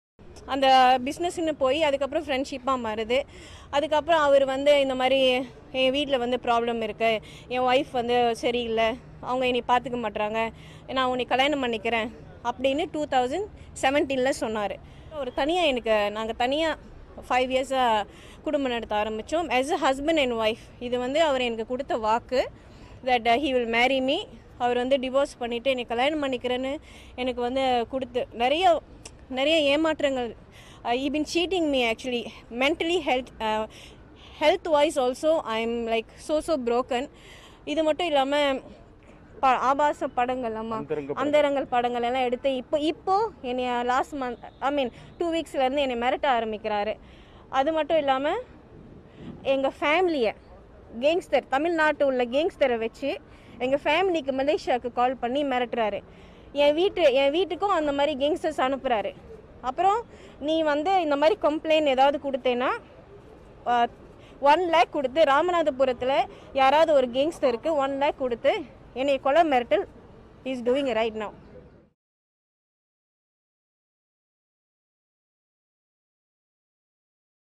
Actress Chandini Reveals Original Character Of AIDMK Minister Manikandan | Mana TFI
Actress Chandini Reveals The Real Character Of AIDMK Minister Manikandan |